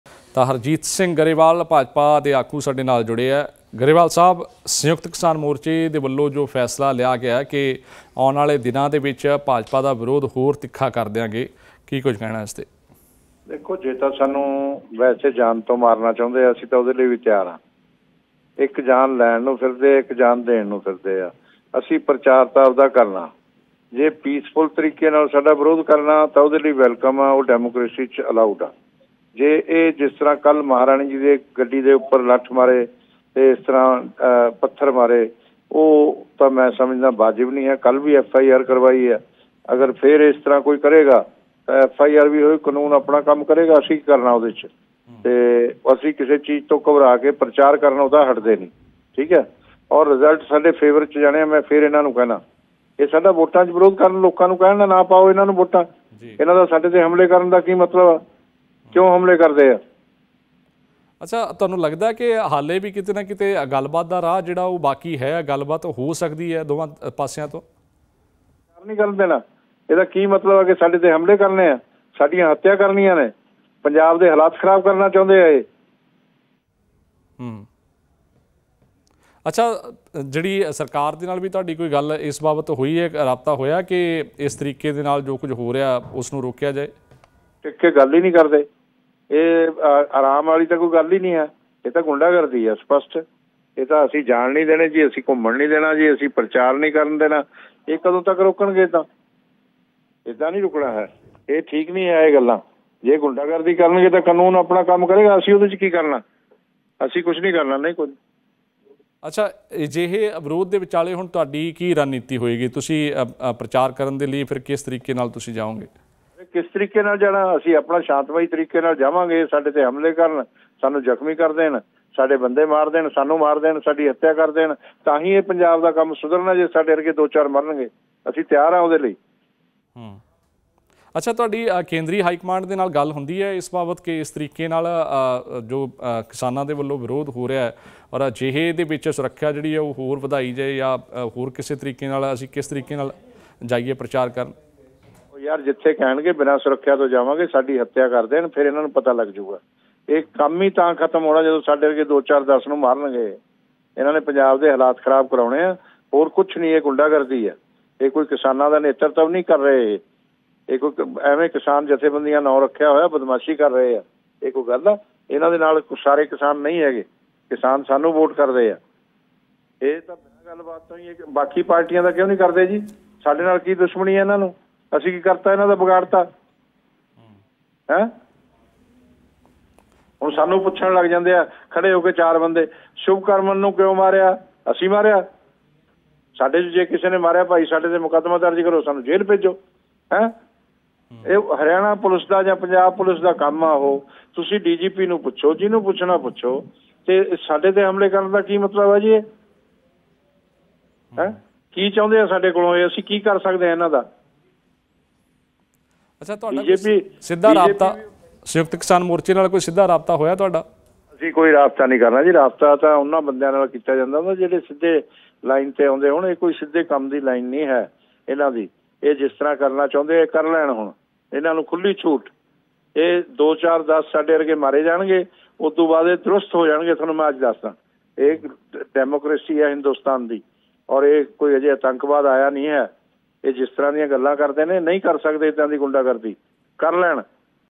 ਵਿਰੋਧ ਕਰ ਰਹੇ ਕਿਸਾਨਾਂ 'ਤੇ ਬੀਜੇਪੀ ਆਗੂ ਹਰਜੀਤ ਗਰੇਵਾਲ ਦਾ ਜ਼ੁਬਾਨੀ ਵਾਰ| Daily Post PHH
ਵਿਰੋਧ ਕਰ ਰਹੇ ਕਿਸਾਨਾਂ 'ਤੇ ਬੀਜੇਪੀ ਆਗੂ ਹਰਜੀਤ ਗਰੇਵਾਲ ਦਾ ਜ਼ੁਬਾਨੀ ਵਾਰ
 'ਭਾਜਪਾ ਨੂੰ ਵੋਟਾਂ  ਨਾ ਪਾਓ ਪਰ ਸਾਡੇ 'ਤੇ ਹਮਲੇ ਕਰਨ ਦਾ ਕੀ ਮਤਲਬ ?'
'ਜੇ ਸਾਨੂੰ ਜਾਨ ਤੋਂ ਮਾਰਨਾ ਚਾਹੁੰਦੇ ਨੇ ਤਾਂ ਅਸੀਂ  ਉਹਦੇ ਲਈ ਵੀ ਤਿਆਰ'

airtel- 567
DEN- 533
hathway- 768